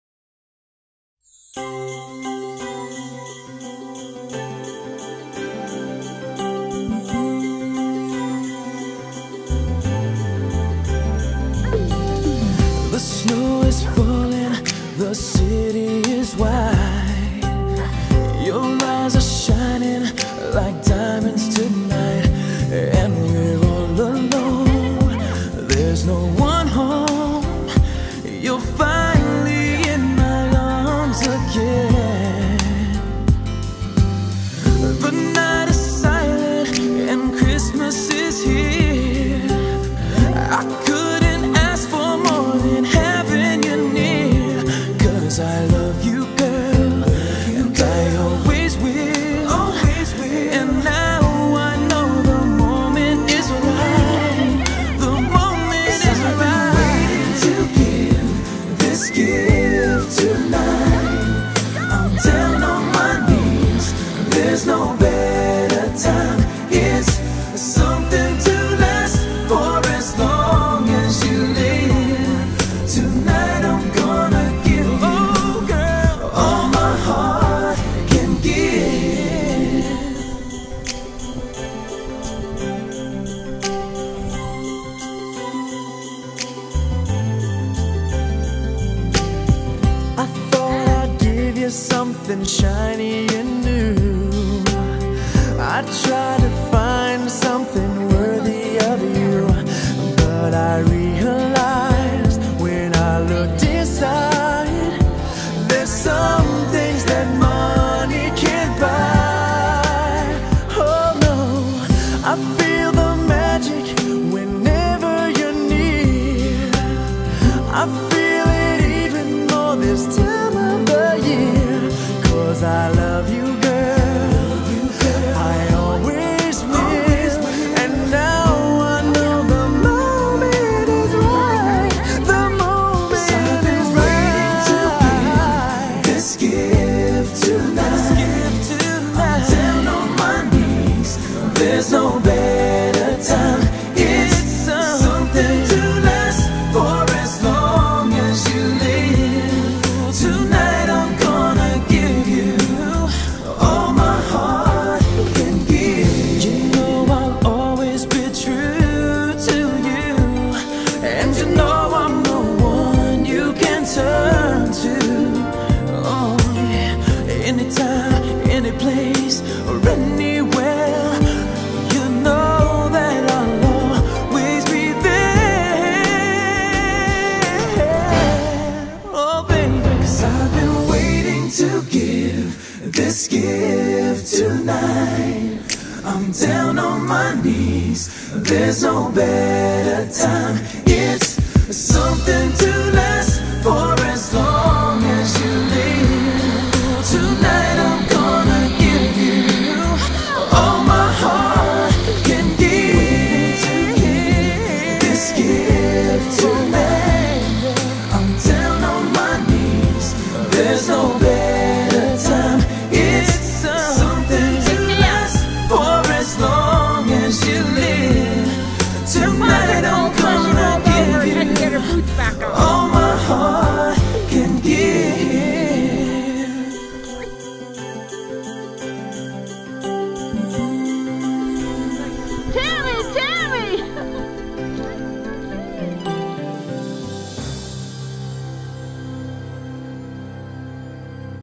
Snow Shoe 2010
Hiking the airport loop in the frigid 4 degree temps in Flagstaff, December 2010